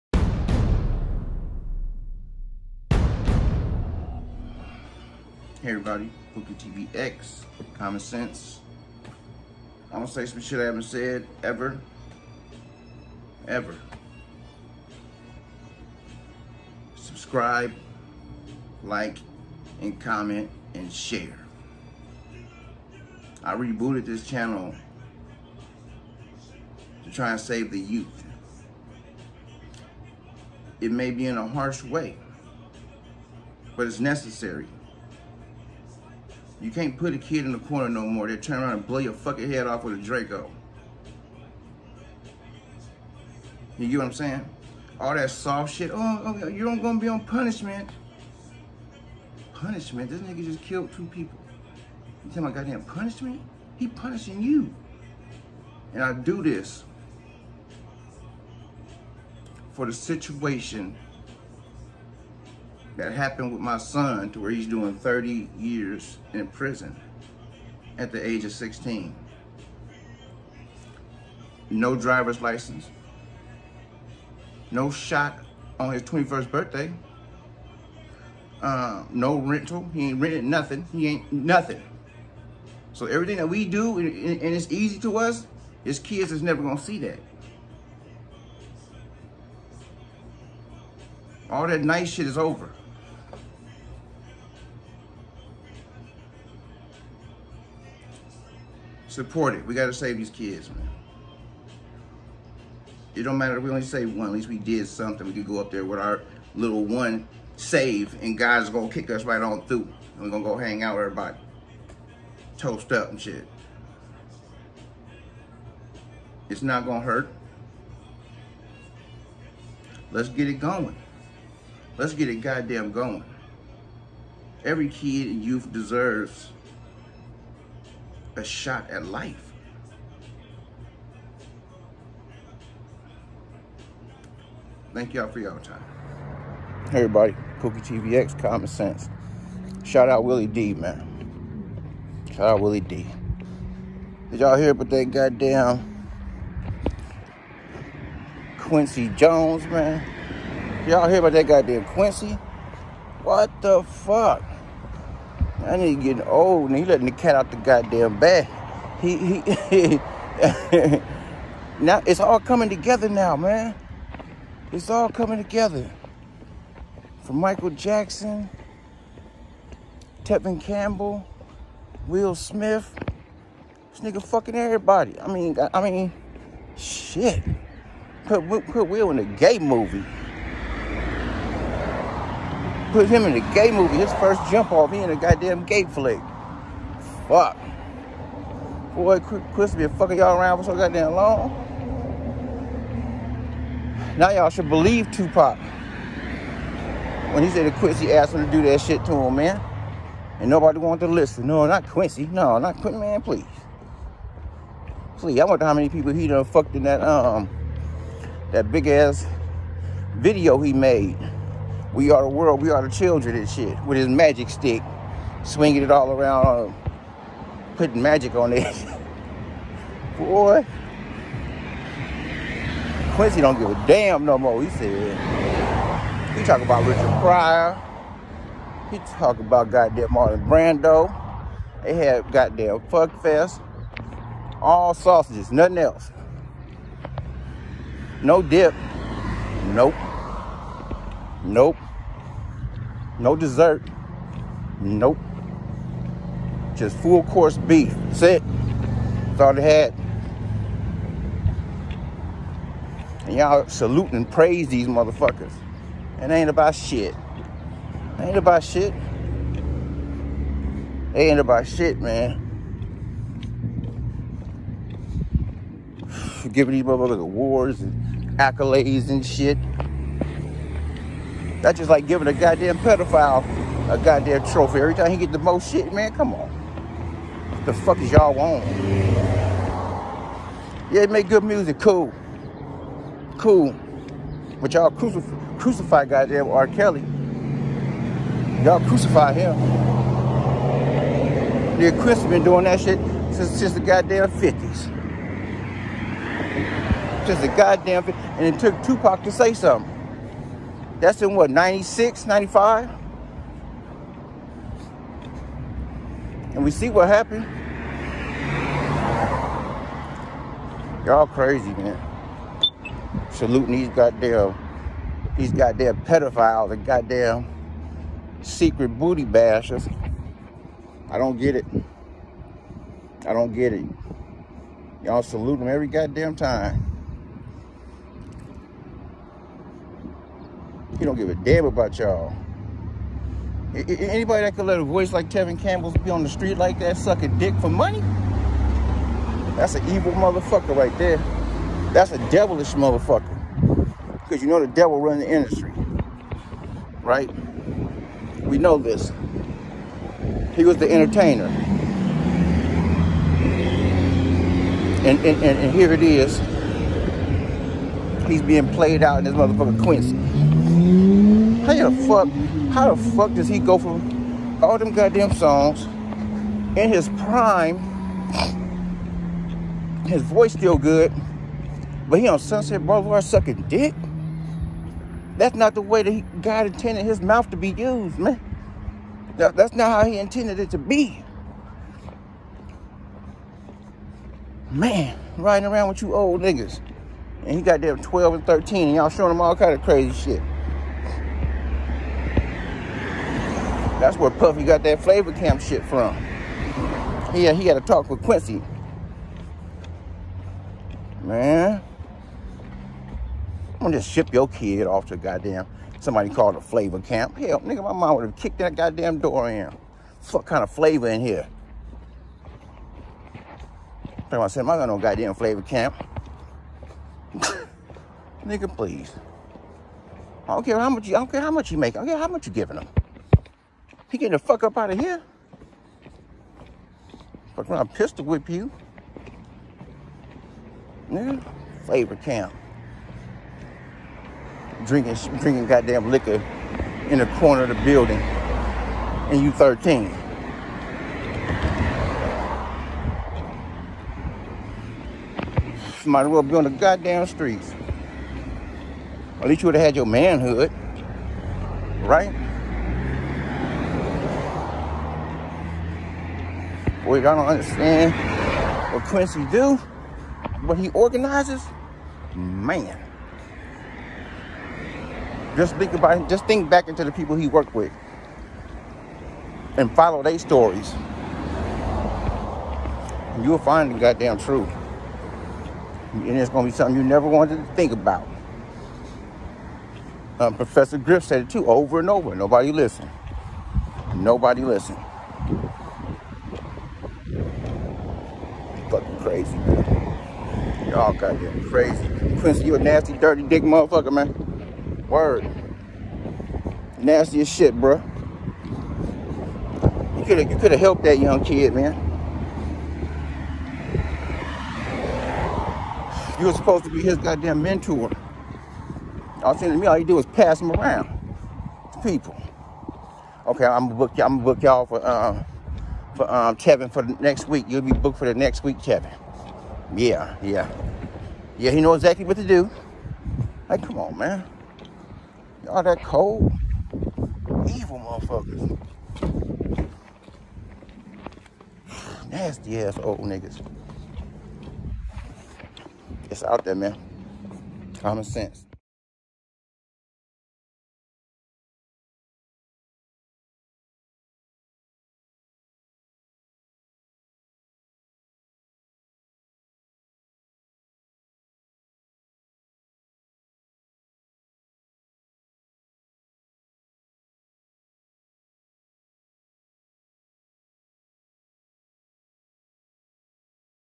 “QUINCY JONES” THE LEGEND OF HIDING DIRTY DISGUSTING SECRETS SMH 🤦🏾‍♂️ LOL 😂! MAN YALL NEED TO
POOKIETVX COMMON SENSE! “QUINCY JONES” THE LEGEND OF HIDING DIRTY DISGUSTING SECRETS SMH 🤦🏾‍♂️ LOL 😂! MAN YALL NEED TO STOP WORSHIPING THESE CELEBS! YALL PRAISING THEM FOR DOING FOUL ISSHH! THEY DONT GIVE A DAM ABOUT YOU OR YO KIDS AND FAMILY! NEVER WORSHIP A HUMAN BEING! 👿👿👿⏰⏰⏰🤷🏽‍♂️🤷🏽‍♂️🤷🏽‍♂️🤡🤡🤡🤦🏾‍♂️🤦🏾‍♂️🤦🏾‍♂️💯💯💯